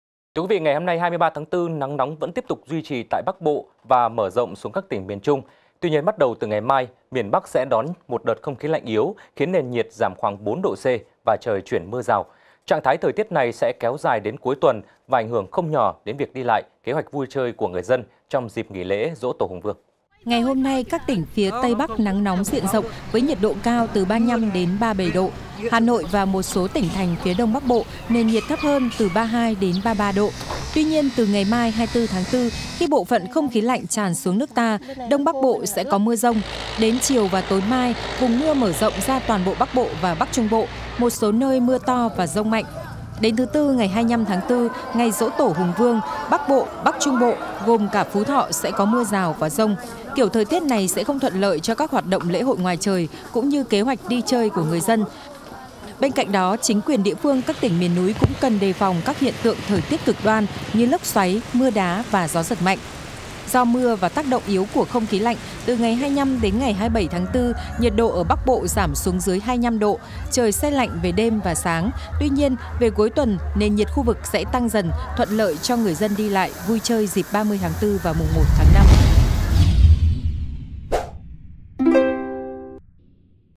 VTC14 | Miền Bắc chuyển mưa rào từ ngày mai
Ngày hôm nay 24/4, nắng nóng vẫn tiếp tục duy trì tại Bắc Bộ và mở rộng xuống các tỉnh miền Trung. Tuy nhiên bắt đầu tư ngày mai, miền Bắc sẽ đón một đợt không khí lạnh yếu khiến nền nhiệt giảm khoảng 4°C và trời chuyển mưa rào. Trạng thái thời tiết này sẽ kéo dài đến cuối tuần và ảnh hưởng không nhỏ đến việc đi lại, kế hoạch vui chơi của người dân trong dịp nghỉ lễ Giỗ tổ Hùng